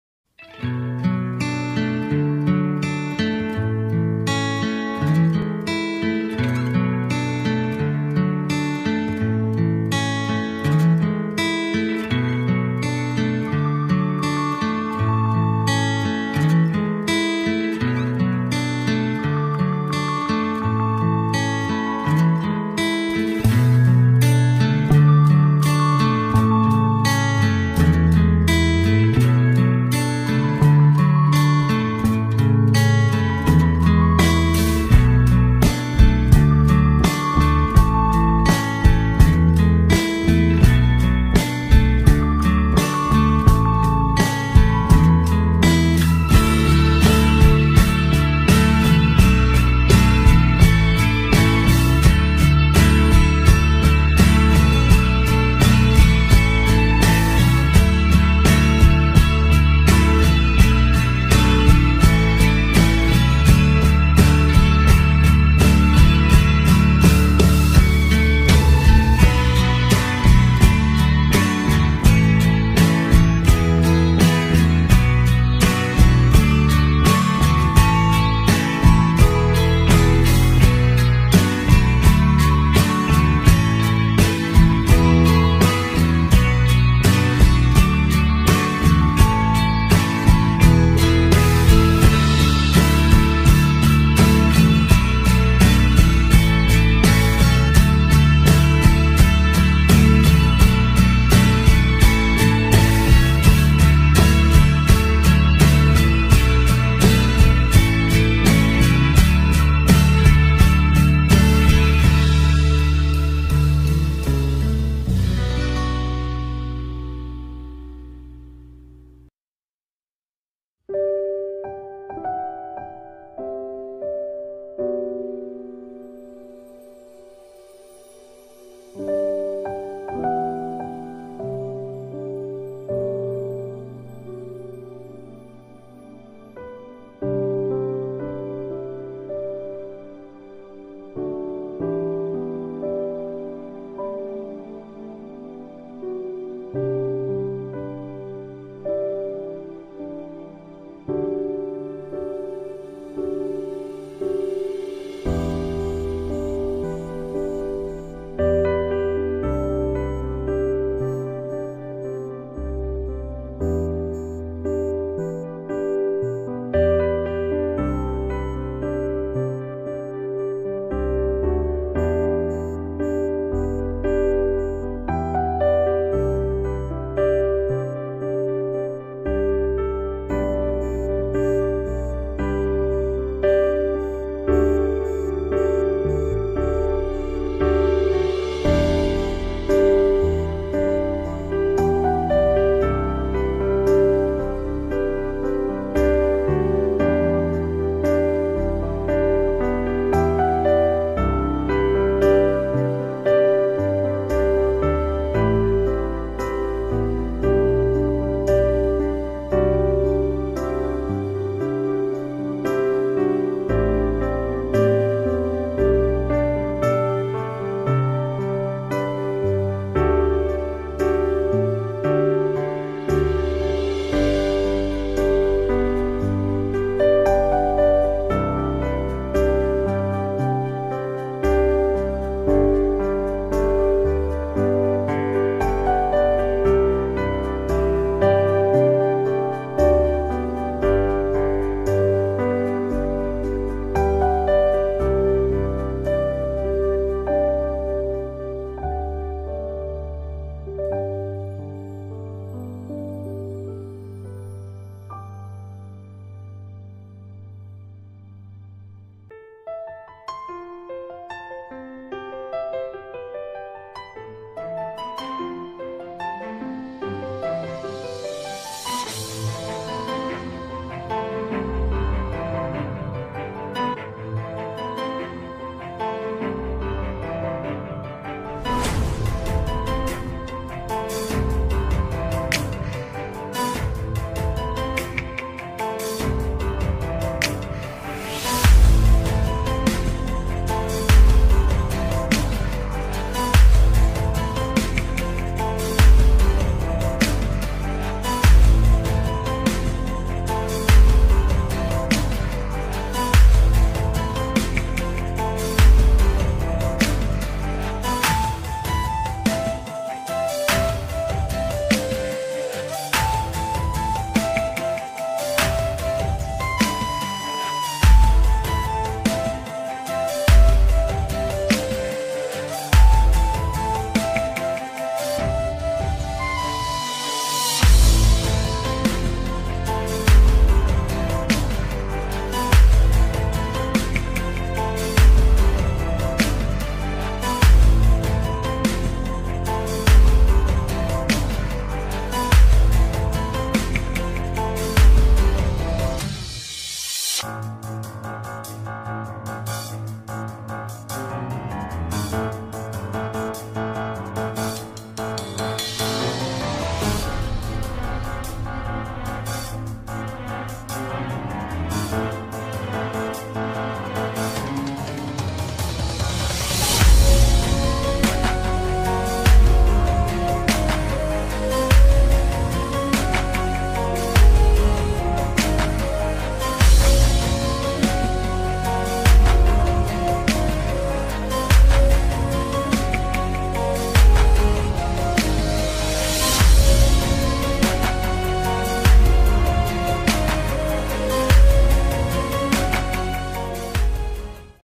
Drawing "St. Pope John Paul II and Our Lady of Guadalupe" | Raizodark
Colored pencils drawing of " St. Pope John Paul II and Our Lady of Guadalupe".
- Colored pencils : Prismacolor premier, Caran d'Ache Luminance
- Paper : Vellum board A3